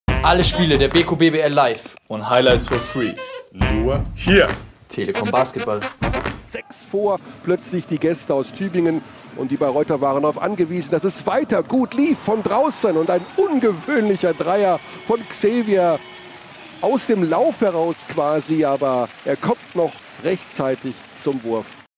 Circus Shot! Jeffrey Xavier mit einem sehr außergewöhnlichen Dreier aus dem Lauf!
Beim knappen Spiel von medi bayreuth gegen die WALTER Tigers Tübingen verwandelt Xavier einen eher ungewöhnlichen Dreier zum Ablauf der Shotclock.